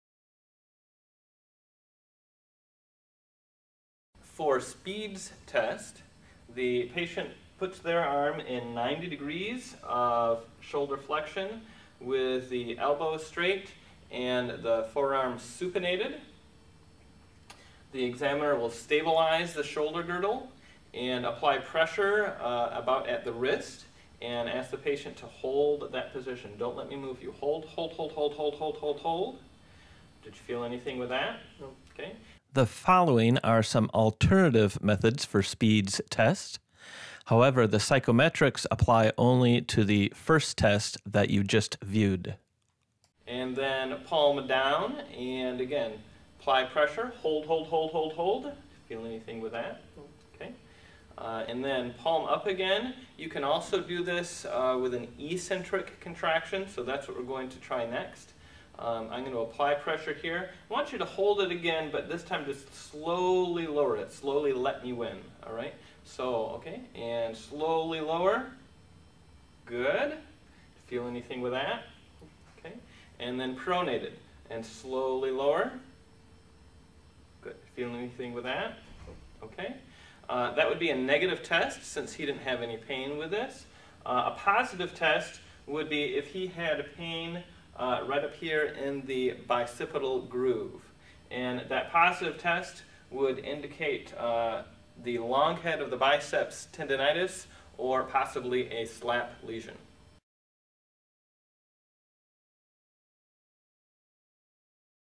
Speeds Test for Long Head of the Biceps Tendinopathy
Speed’s test (for bicipital tendinopathy)
Test Procedure:
• Client actively flexes GH joint to 90°, elbow extended, forearm supinated.  Examiner resists flexion, applying pressure to the distal forearm.

Positive results:
• Pain in the bicipital groove

Negative Results:
• No pain

Implications of positive test result:
• SLAP lesion or bicipital tendinopathy

Psychometric Properties for LH biceps tendinopathy:  (Cleland et al., 2022)
Sensitivity: 0.49 Specificity: 0.67
LR+: 1.48  LR-: 0.76
(Note that the above numbers indicate that Speed's is minimally helpful for diagnosing LHB tendinopathy)

Psychometric Properties for SLAP: (Cleland et al., 2022)
Sensitivity: 0.32 Specificity: 0.61
LR+: 0.80  LR-: 1.11
*(Note that the above numbers indicate that Speed's is worse than worthless for diagnosing a SLAP lesion)